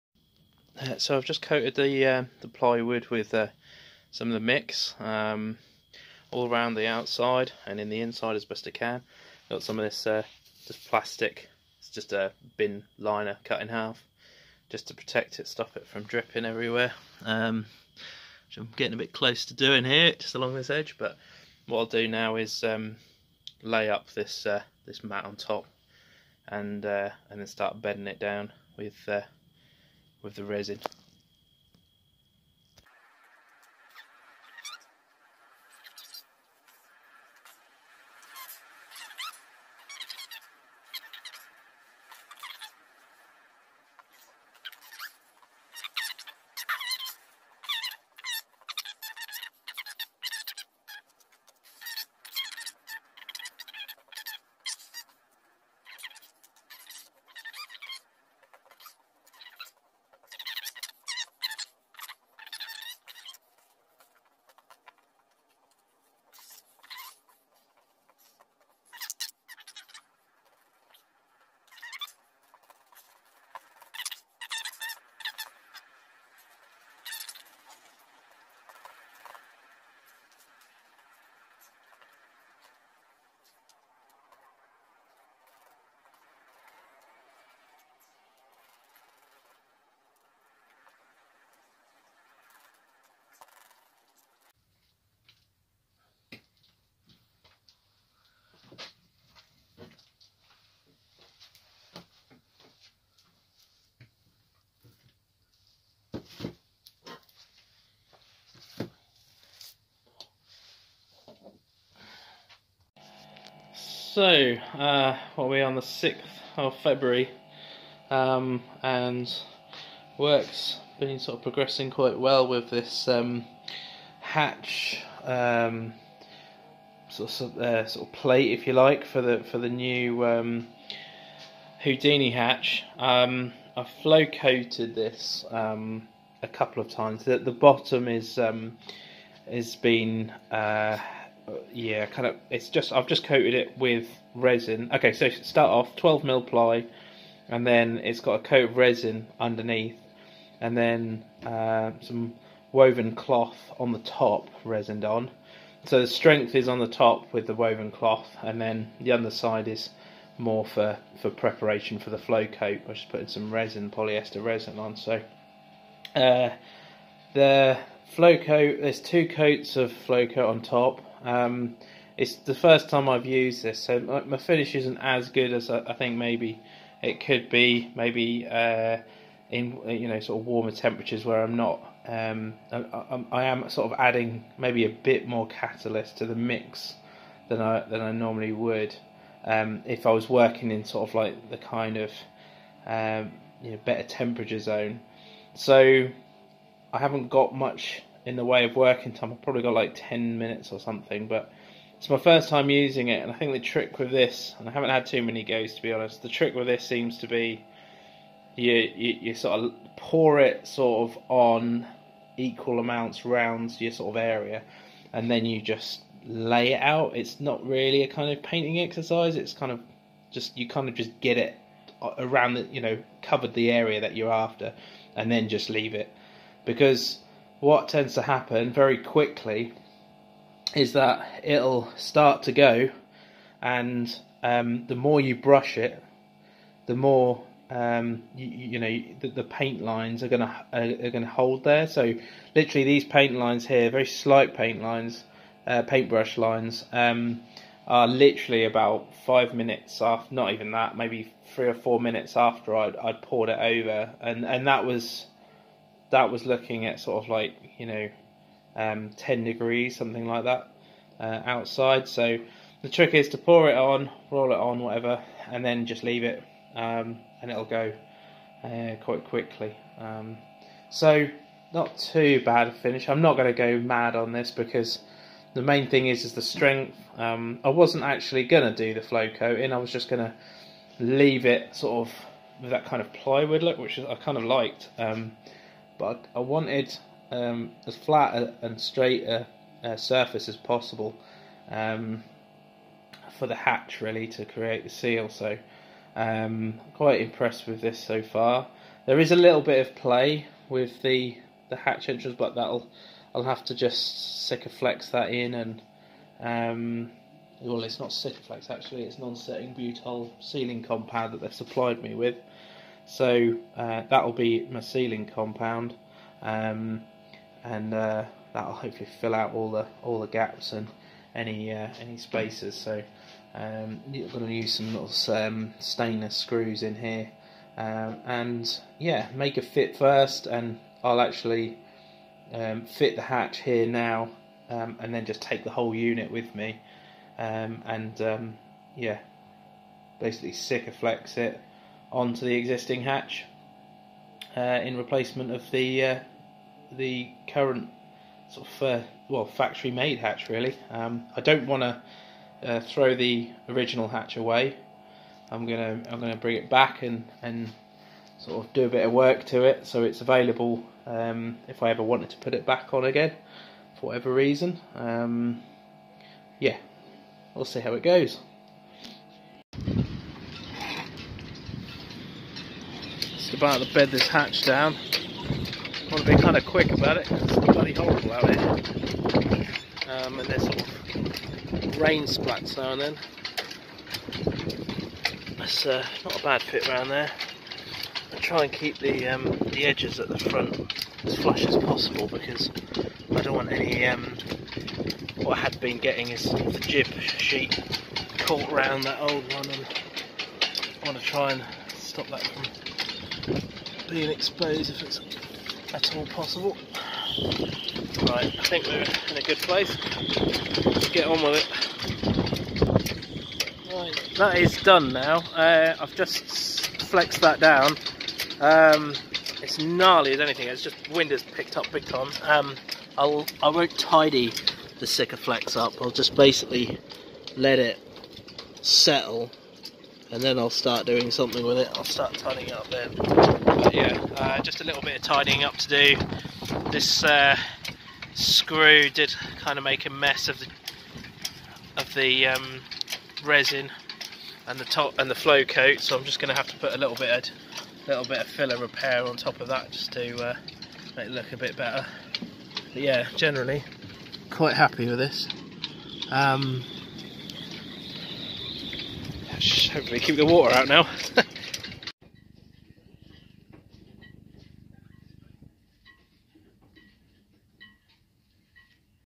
Installing new foredeck hatch | Houdini Hatch | Hurley 22
I go to town on my new forward deck hatch by installing a Houdini hatch into a reinforced plywood panel fit to the size of my deck opening.